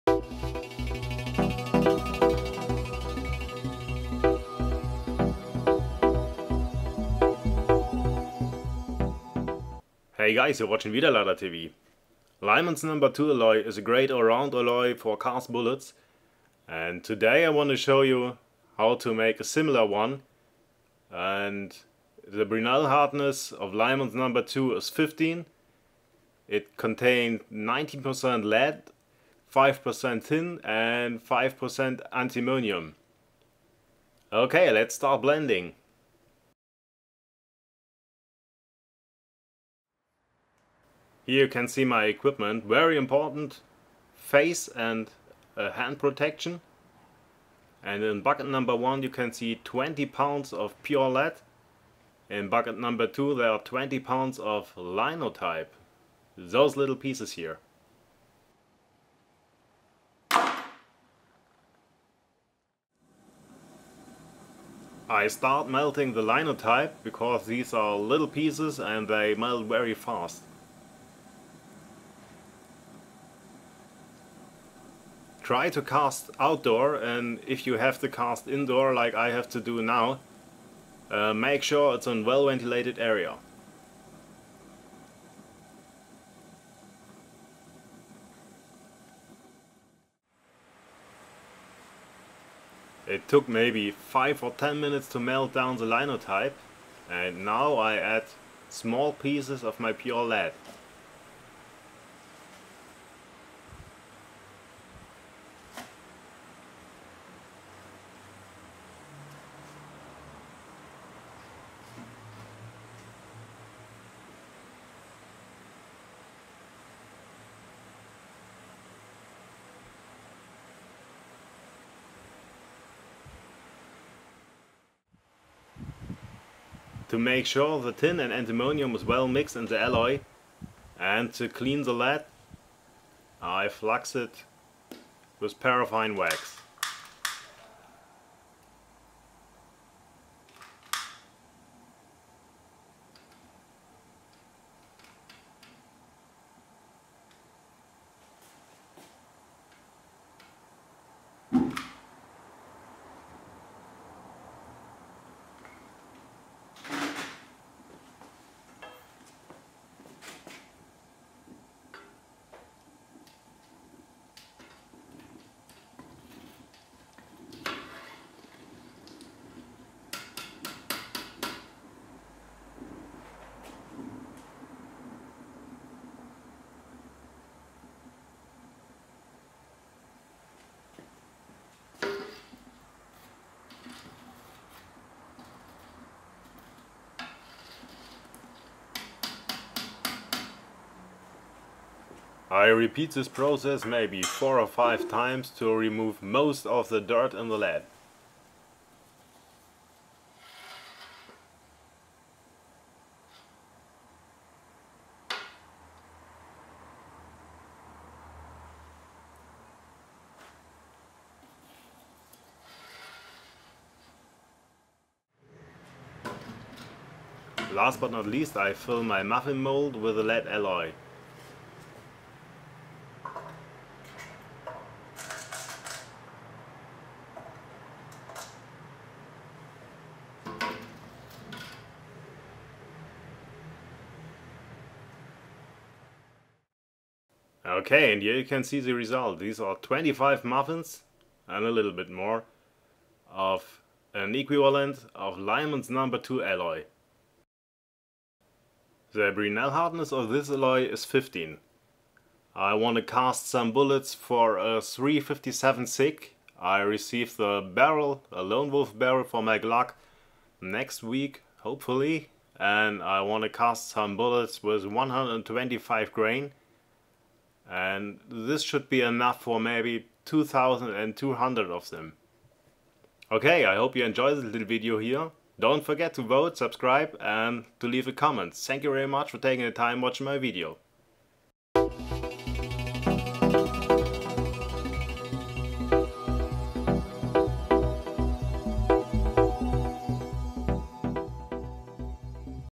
How to make #2 alloy for cast bullets
In this Video I Show you how to blend different lead sources to make an alloy similar to Lyman #2. The brinell hardness is 15 and I wanna use it to cast some lead bullet for the .357 SIG. This recipe is an very cheap way to make hard cast bullets for hot rounds.

wiederladertv, cast bullets, #2 alloy, lyman, 357 sig, reloading, boolitz, lead, alloy